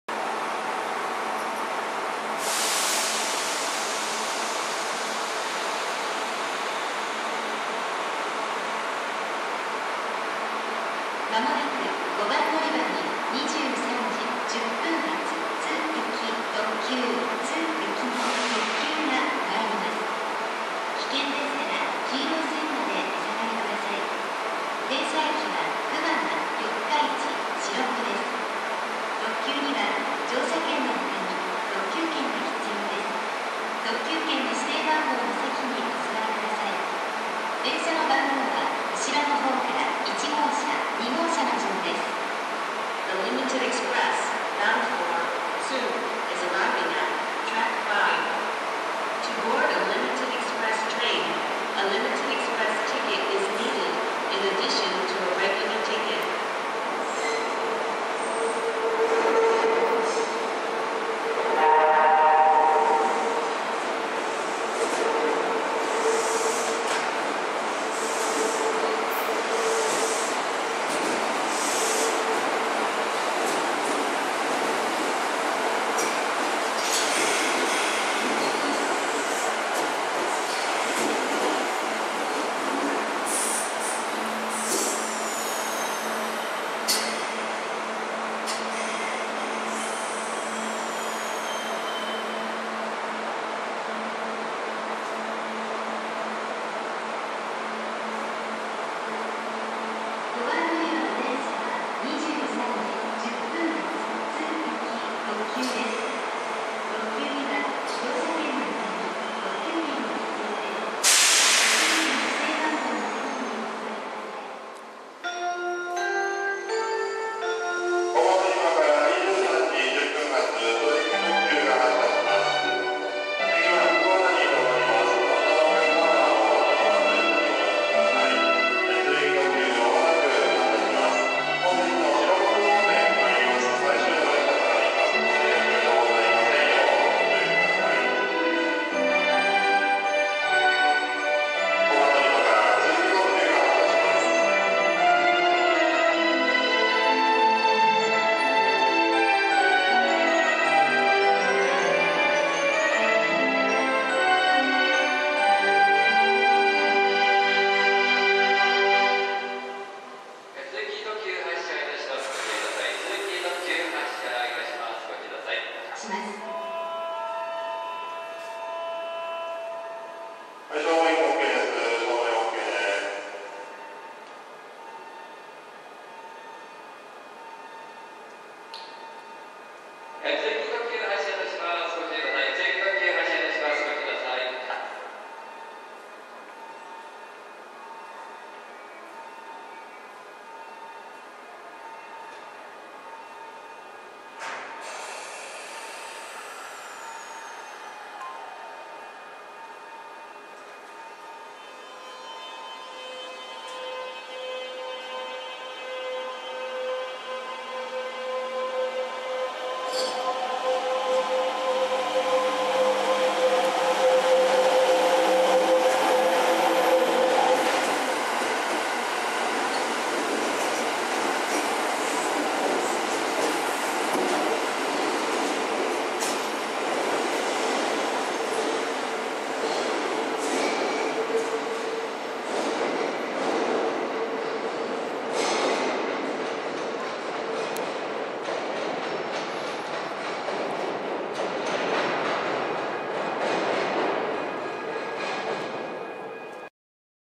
近鉄 9313・津 AS27+AS09
2018/11/29(木)
近鉄名古屋より
名古屋線では珍しいAS新+旧の組成が発生しました。